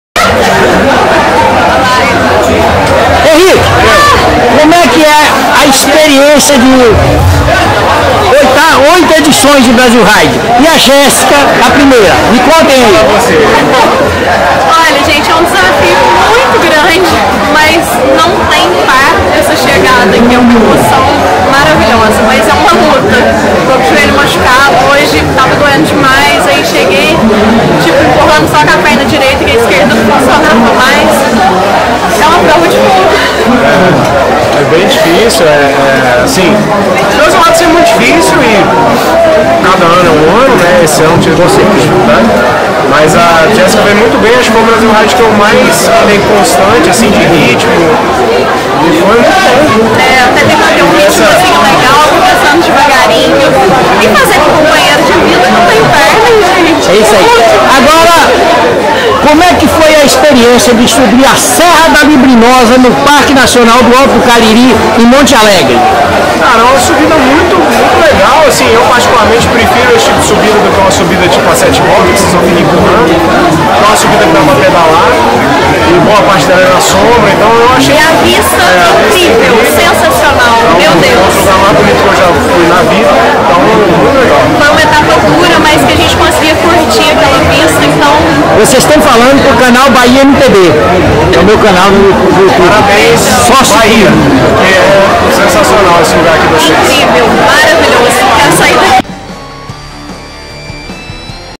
Última etapa da Brasil Ride 2021
O Canal Bahia MTB conversa com Henrique Andrade e Jéssica o casal 20 do Mountain Bike